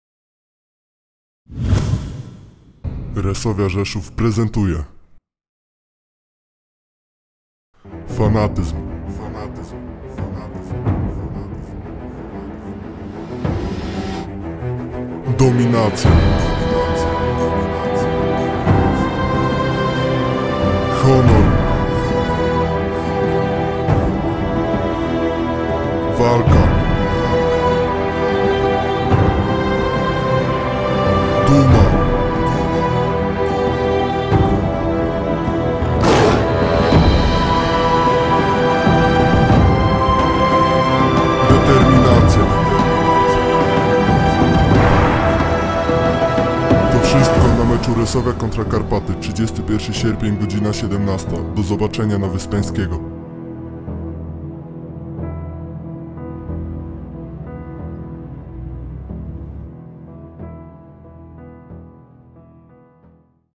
Zapowiedź: Resovia Rzeszów - Karpaty Krosno ( 31.08.2013, g.17:00 )
Hit Rundy!
Resovia Rzeszów vs Karpaty Krosno godz. 17:00
Stadion Resovii, ul. Wyspiańskiego 22